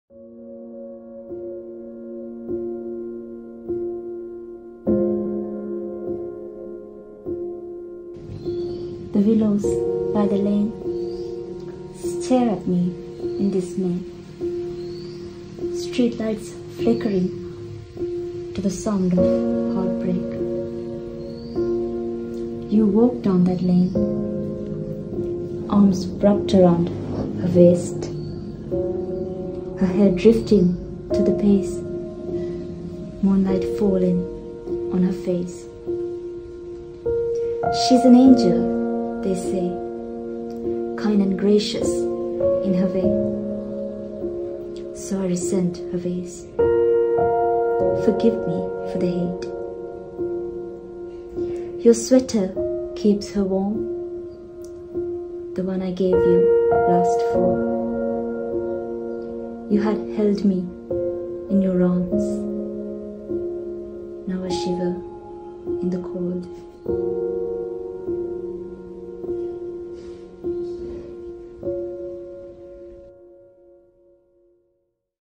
The Sweater
💔 Experience the bittersweet tale of a girl haunted by lost love 💔

Watch as delicate steps intertwine with poetic verses, painting a vivid portrait of a girl's shattered heart. Feel the weight of her emotions as she dances to a melancholic melody, each movement resonating with the pain of a love that slipped through her fingertips.

Let the power of words and visuals captivate your senses, reminding you of the fragility of love and the lingering traces it leaves behind.

Watch "The sweater" and embrace the poignant poetry of a broken heart. Together, we can find solace in the beauty of shared stories.